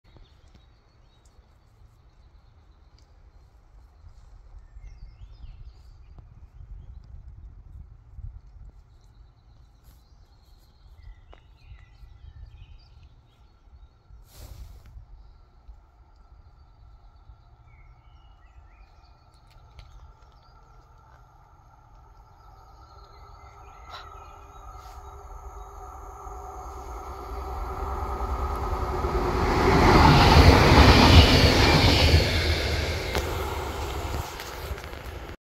(FAIL!!!) Järnvägsövergång Borås (Västergatan) / BROKEN RAILROAD CROSSING IN SWEDEN
I SAW SO DISAPPOINTED!!! THAT IT WAS BROKEN AND NOT SOUNDED!!!
THIS IS DANGEROUS TOO!!!